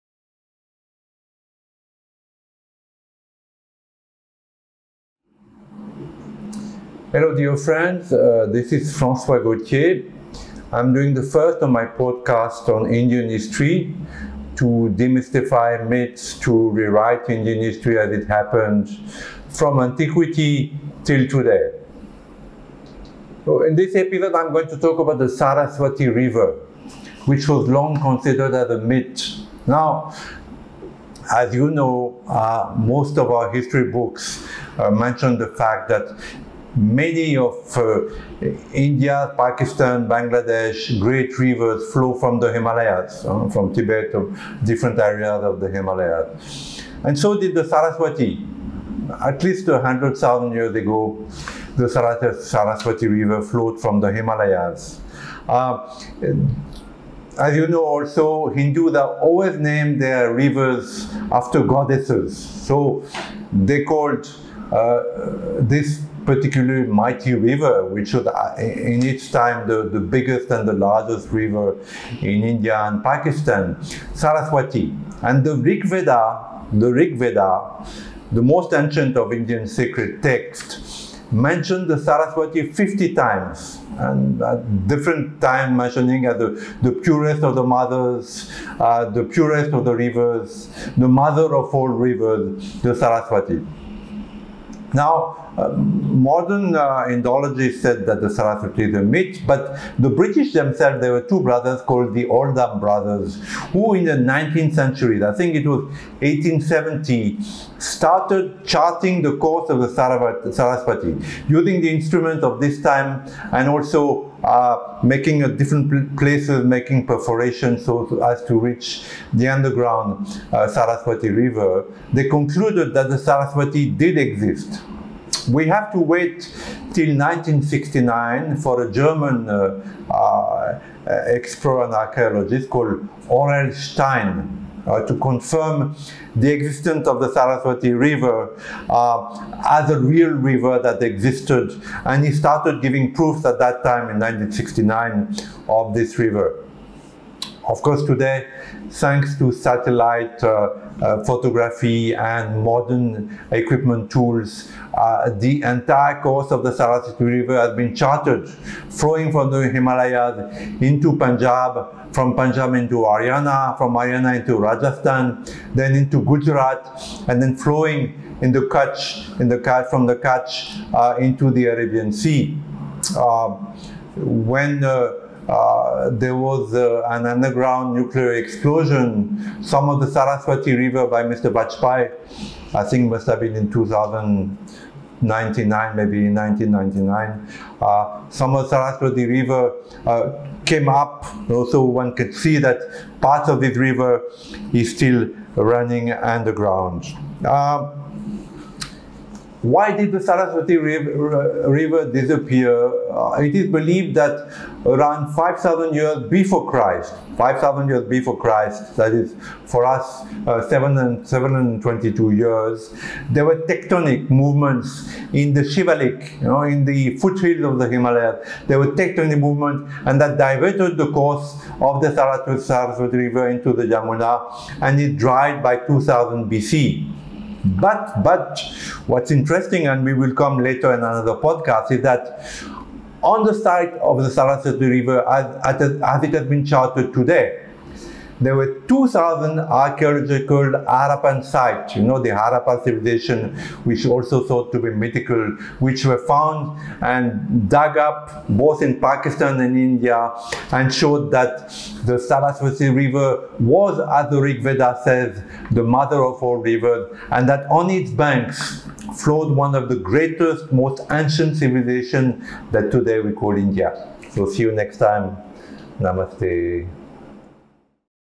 FIRST PODCAST: WHAT IS THE TRUTH ABOUT THE SARASWATI RIVER?
Francois Gautier starts a series of vlogs that rewrite History of India as IT HAPPENED, from Antiquity till today.